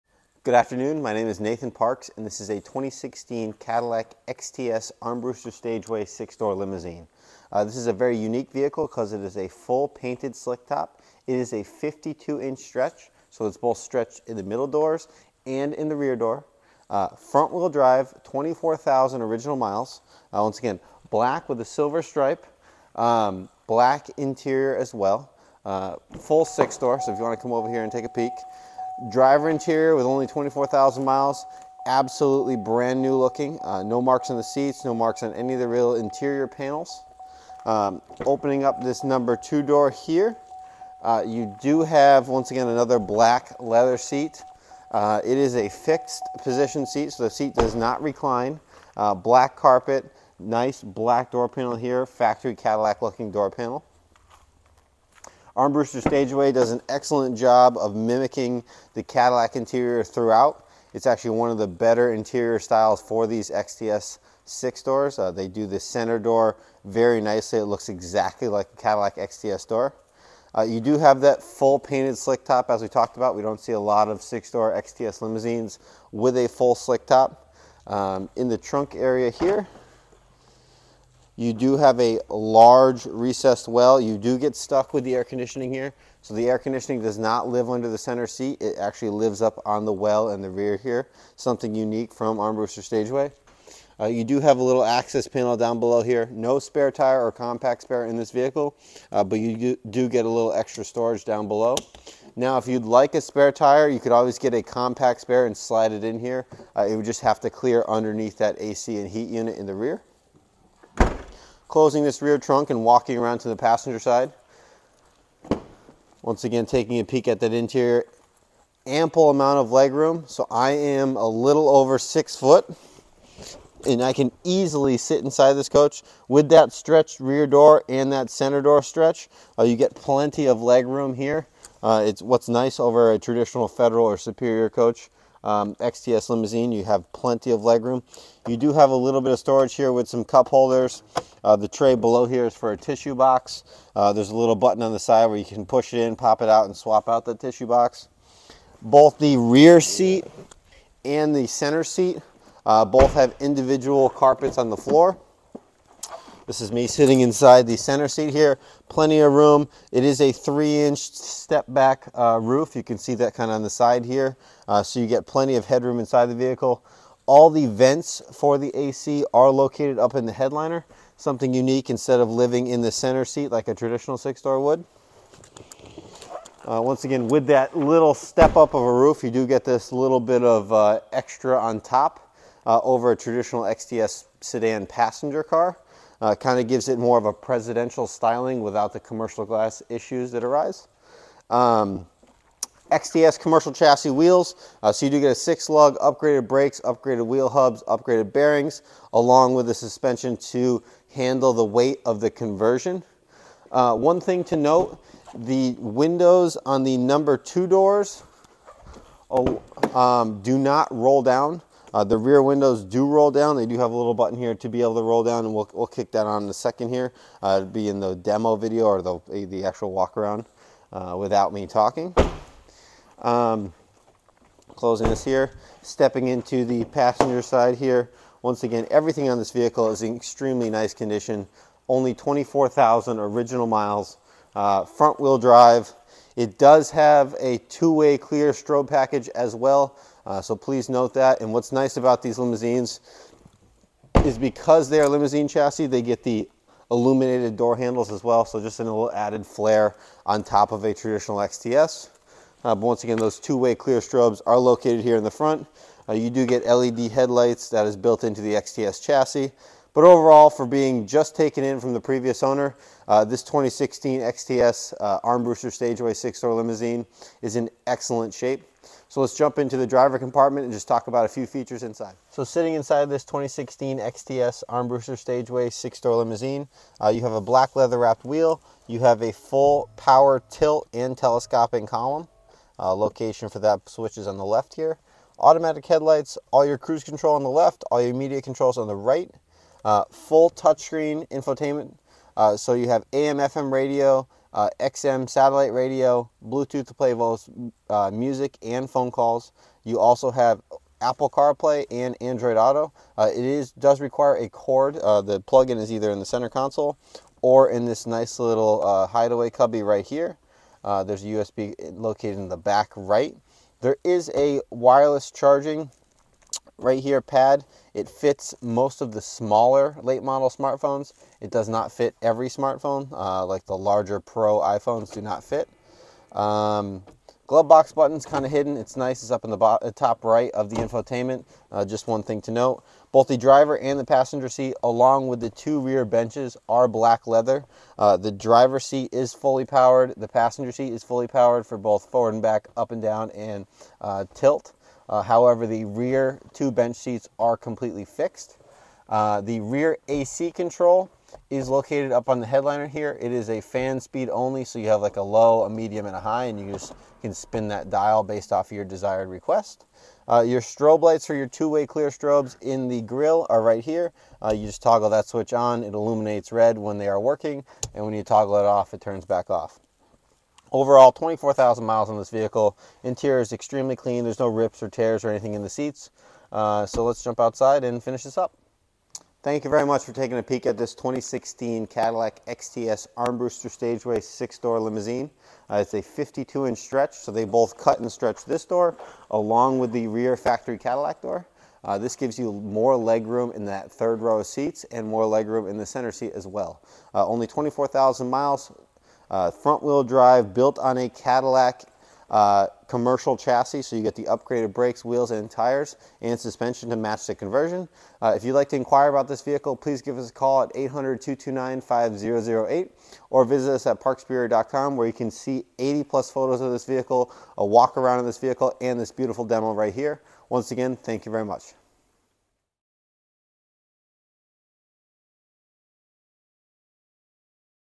2016 Armbruster Stageway Cadillac XTS 6-Door Limo DEMO (G9550324)
JUST IN!! Fully reconditioned and ready for immediate delivery. This 6-Door Limousine features increased interior legroom allows maximum comfort to your families, 2-Way Strobe-Clear, Alloy Wheels, Back Up Camera, , CUE Information and Media Control System, Keyless Start (Push Button), Painted Roof "Slick Top" and SIRIUS Satellite Radio. Garage kept and well cared for, this Armbruster limousine would be a great addition to any funeral home or livery service.